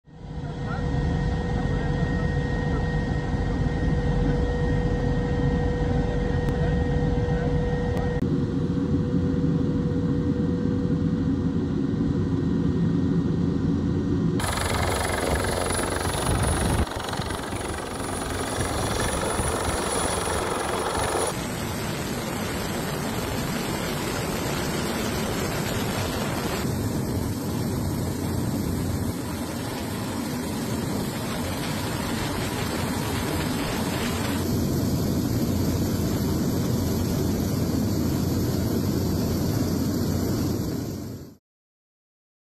Aerials of volcano erupting in southwest Iceland
Aerial images of volcano erupting and spewing lava on the Reykjanes peninsula 3km north of Grindavik in southwest Iceland.